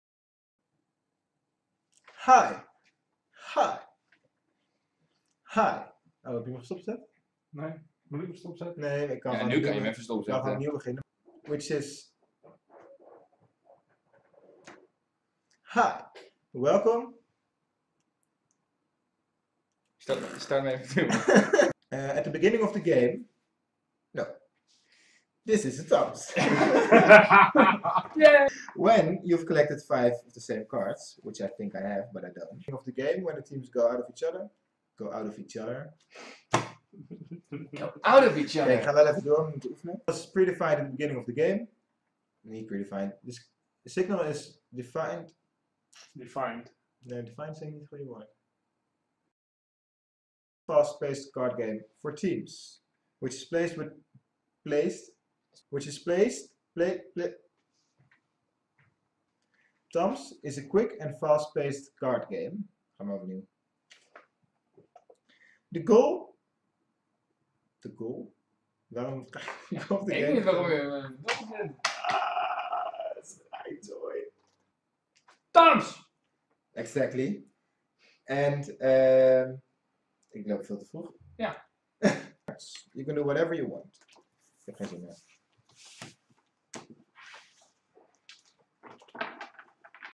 Bloopers Tamps HowToPlay
We tried to make an How to Play movie for the card game of Tamps. It turned out to be a little struggle.

Greets, The Tamps Team